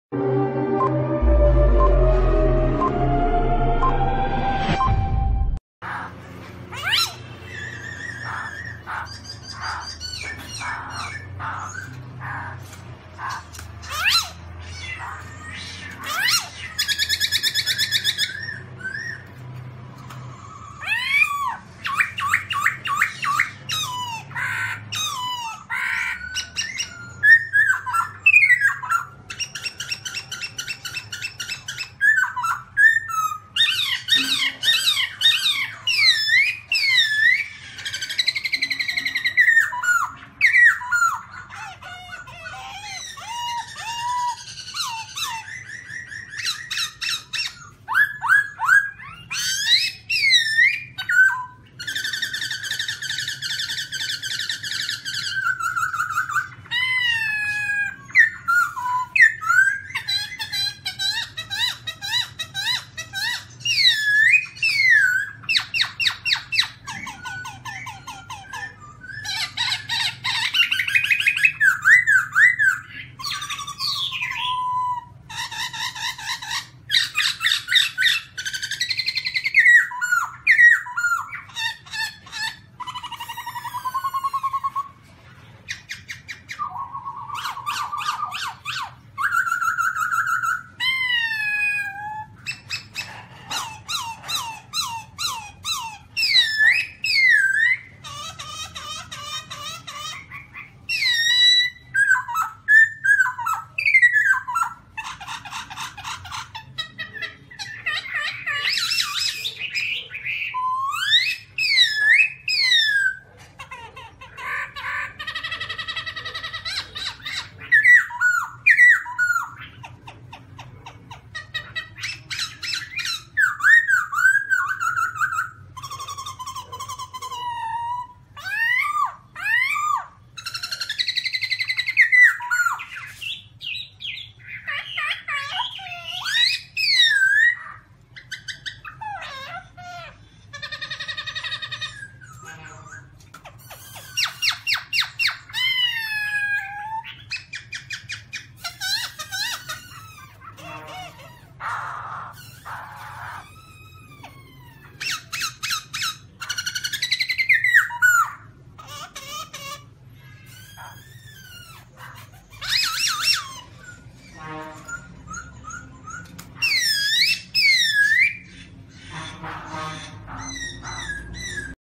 Garrulax chinensis - Birds singing voice well on the mountainrocky - Birdsong 2016
- The black-throated laughingthrush (Garrulax chinensis) is a species of bird in the Leiothrichidae family. It is found in Cambodia, China, Laos, Myanmar, Thailand, and Vietnam. Its natural habitats are subtropical or tropical moist lowland forests and subtropical or tropical moist montane forests.